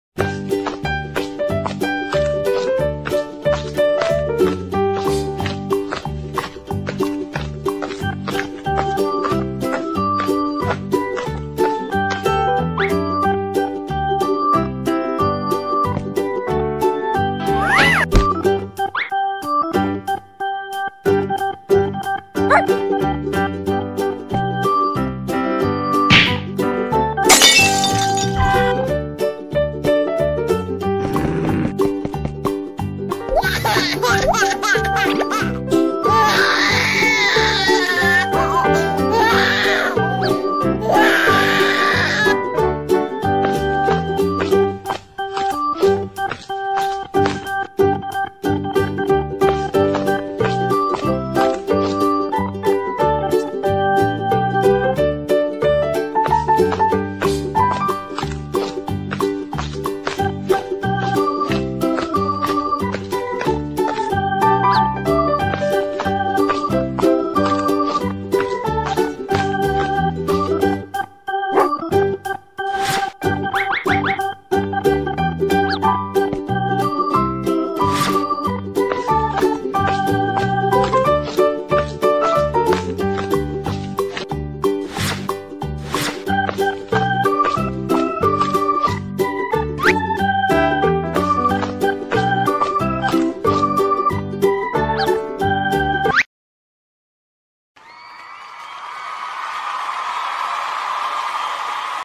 Funny dogs cartoon for children 2018 - Dog's piggy bank
A very funny video cartoon about a pet dog that becomes friends with children.

Cartoon Dog created by Jamie Sale, Cartoonist for hire